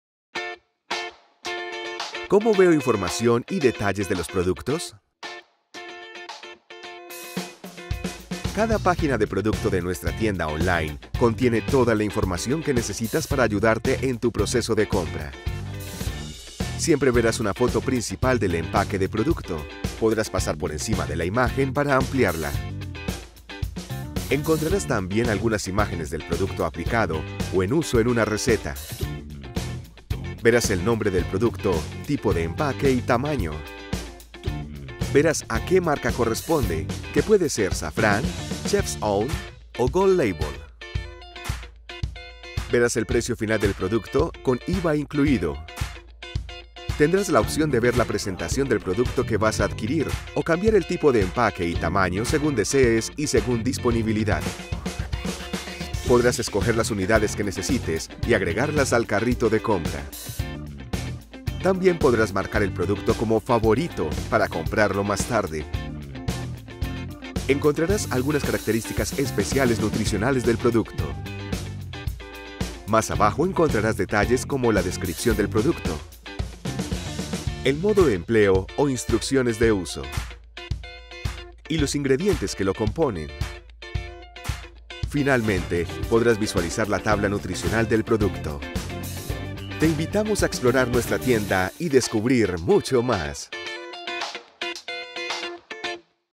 Cómo ver información y detalles del producto Custom Culinary
Explora toda la información que podrás encontrar sobre nuestros productos en la tienda online Custom Culinary